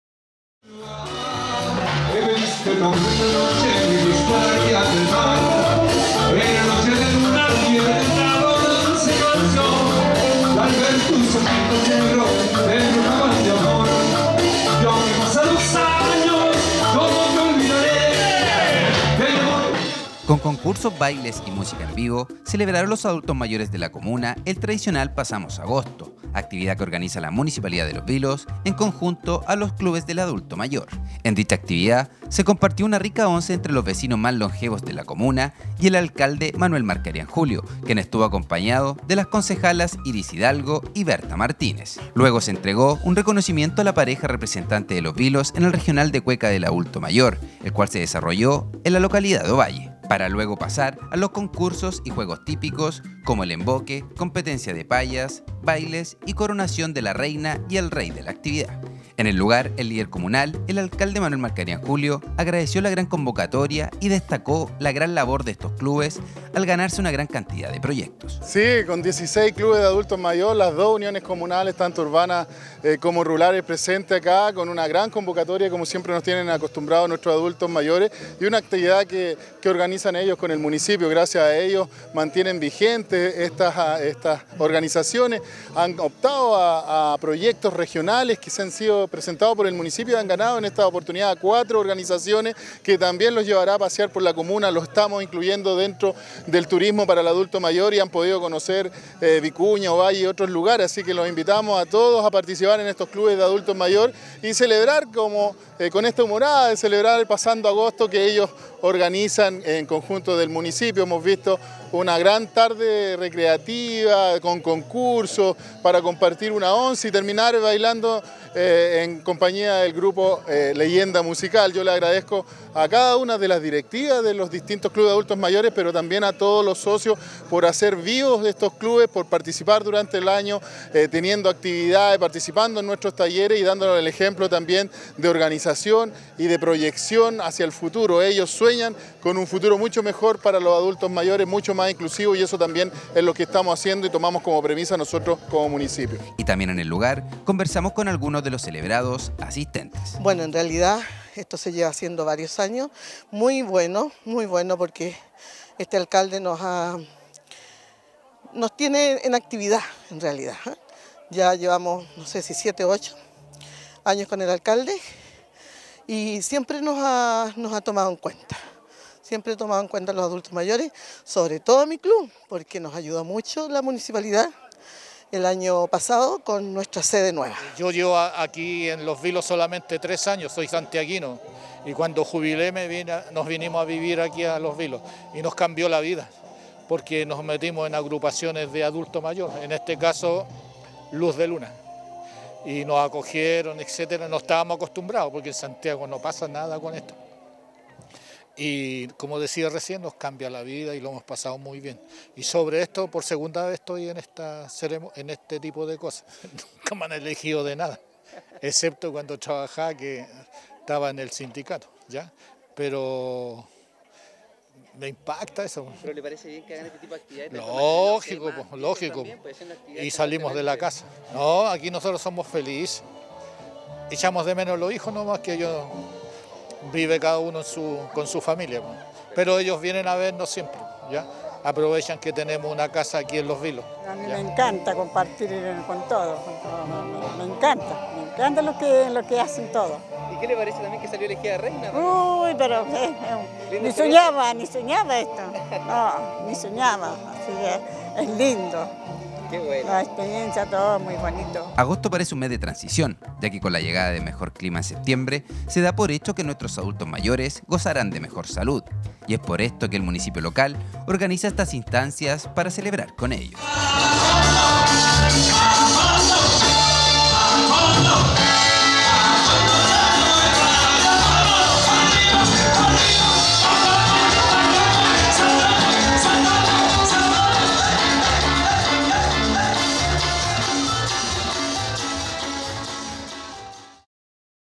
Adultos mayores de la comuna celebraron el tradicional “Pasamos Agosto”
Con concursos, bailes y música en vivo, celebraron los adultos mayores de la comuna el tradicional “Pasamos Agosto”, actividad que organiza la Municipalidad de Los Vilos, en conjunto a los clubes del adulto mayor.
En dicha actividad, se compartió una rica once entre los vecinos mas longevos de la comuna y el alcalde, Manuel Marcarian Julio, quien estuvo acompañado de las concejalas, Iris Hidalgo y Berta Martínez. Luego se entregó un reconocimiento a la pareja representante de Los Vilos, en el regional de cueca de adulto mayor desarrollado en Ovalle, para luego pasar a los concursos y juegos típicos, como el emboque, competencia de payas, bailes y coronación de la reina y rey de la actividad.
Agosto parece un mes de transición, ya que con la llegada de mejor clima en septiembre, se da por hecho que nuestros adultos mayores gozaran de mejor salud, y es por esto que el municipio local organiza estas instancias para celebrar con ellos.